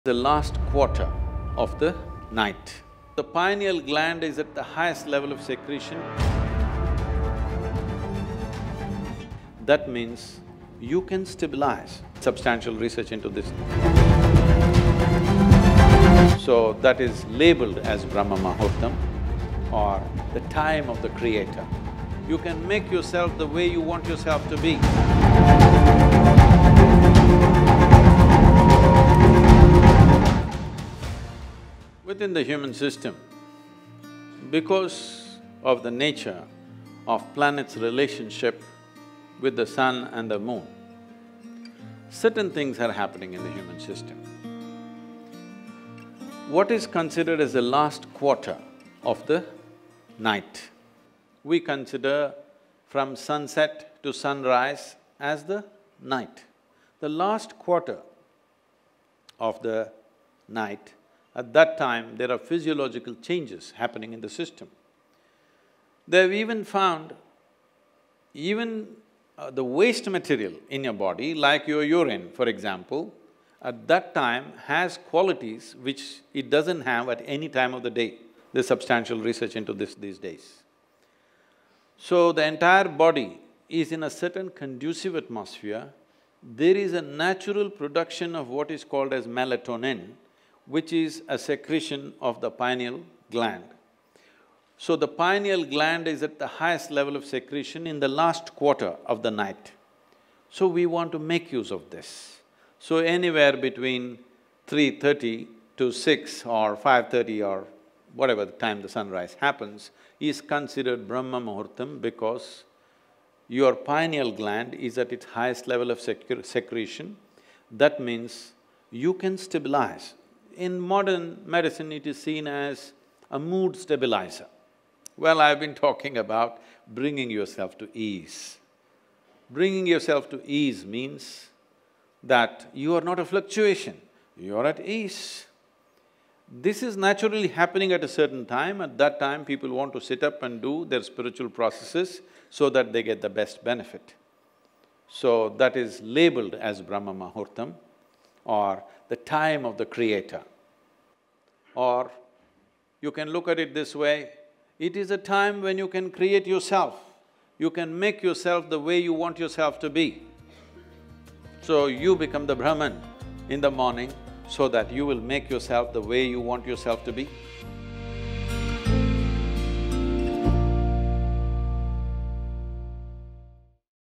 Special Benefits of Waking up Early | Melatonin Activation | Sadhguru
Sadhguru looks at the reason behind the peak melatonin secretions of the pineal gland during the last quarter of the night and how one can make use of it to bring stability and ease.

Get 14-day free access to Sadhguru Exclusive, the largest video catalog on Mysticism & Spirituality.

Inner Engineering

Sadhguru app